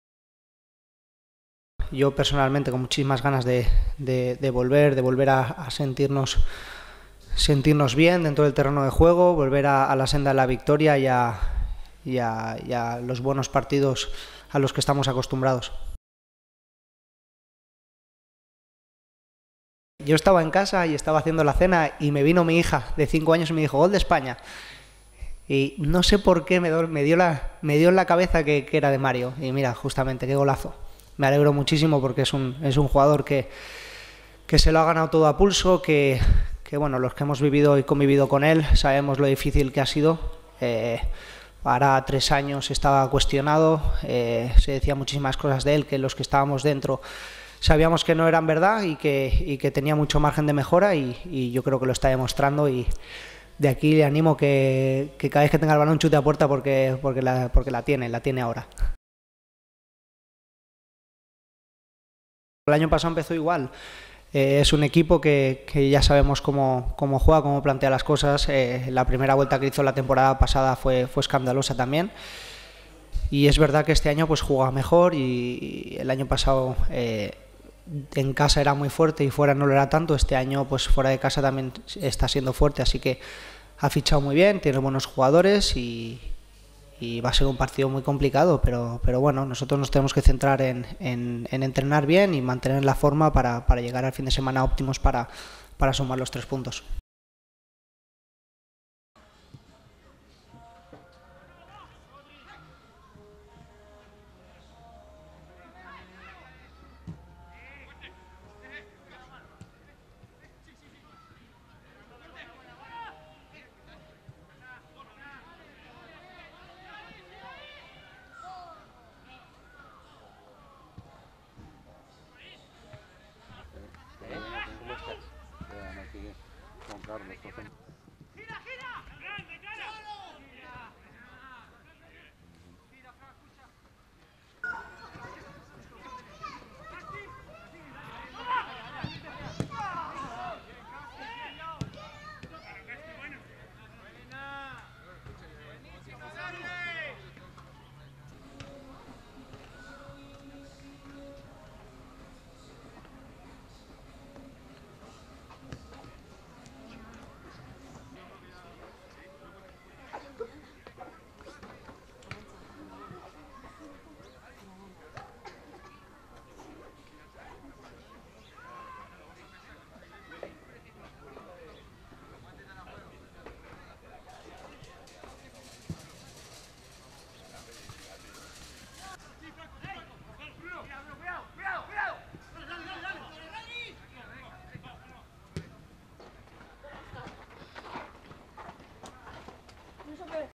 Jaume Costa: “Marcelino sabe sacar el máximo de nosotros"
El lateral izquierdo valenciano del Villarreal, Jaume Costa, se mostró optimista de cara a los próximos partidos del Submarino, en los que los amarillos intentarán quitarse el mal sabor de boca que les dejó la derrota en el Camp Nou ante el FC Barcelona. “Tenemos muchas ganas de volver, sobre todo porque nos fuimos al parón con una derrota dolorosa. Tenemos ganas de volver a sentirnos bien dentro del campo y retomar a la senda de la victoria”, aseguró Jaume Costa.